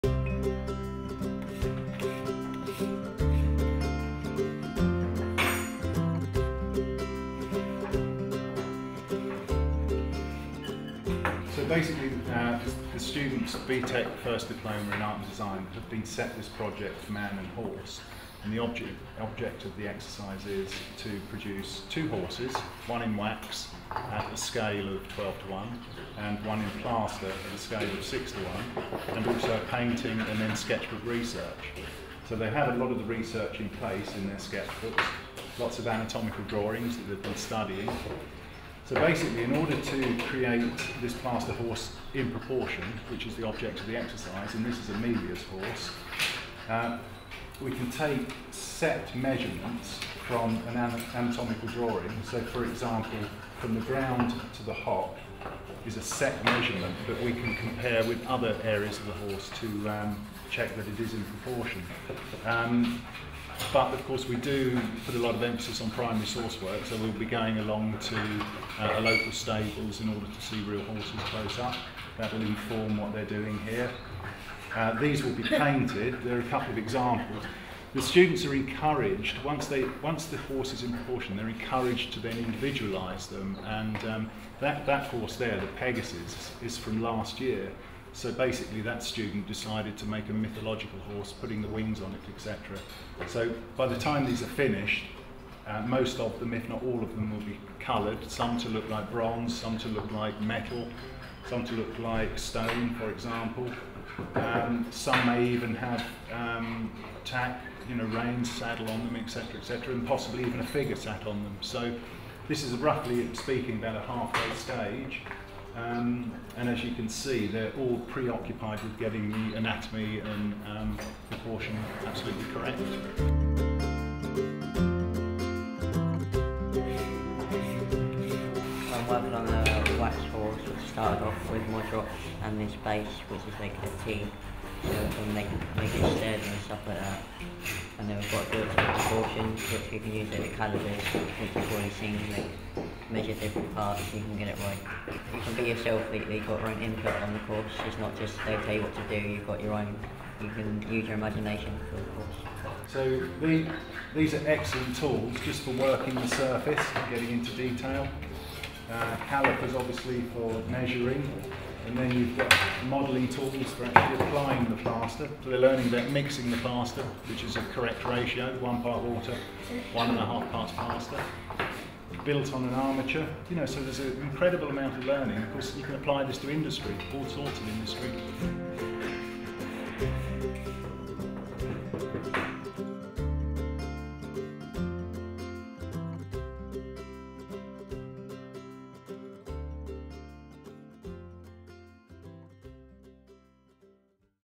'Man and Horse' Art Project at Highbury College Portsmouth
Whatever creative course you study at Highbury, you will always have the freedom to develop your own ideas and skills.